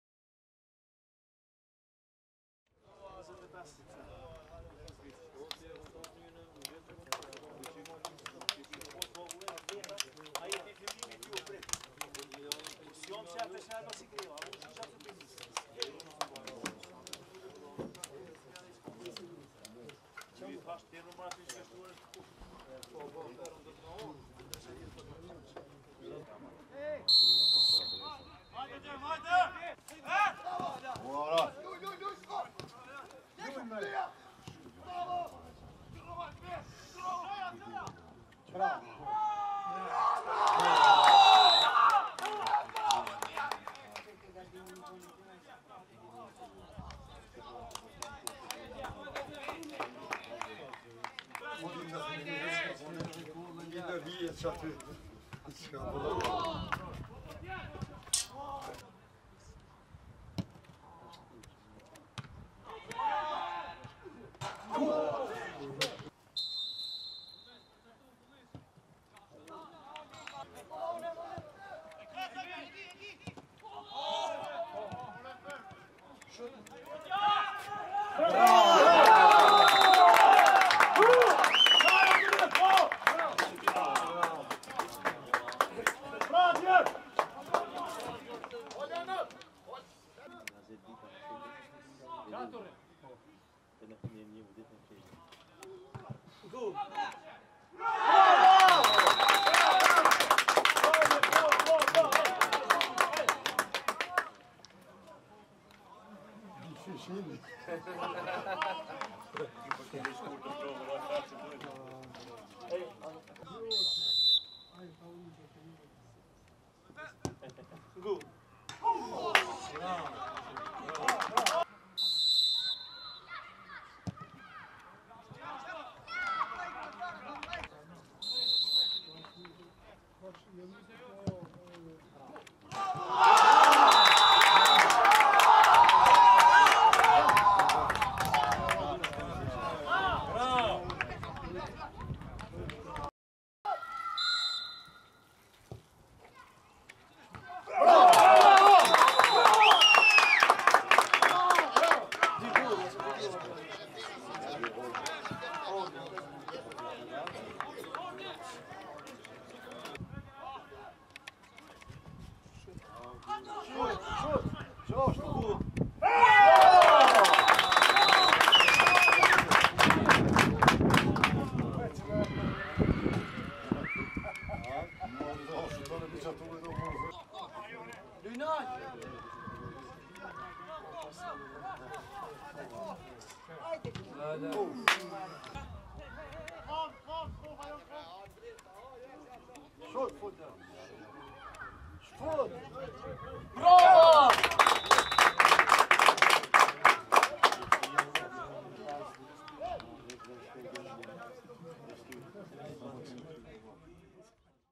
Highlights - KF 2 Korriku 5 - 2 KF Trepca U19 - 22/09/2019
The game highlights between KF 2 Korriku 5 - 2 KF Trepca U19 - 22/09/2019
Some of the best moments during this game held in KF 2 Korriku Stadium.

SWIFT CODE : NLPR XK PR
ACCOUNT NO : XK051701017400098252
Beneficiary Name : GOLDMOUTH G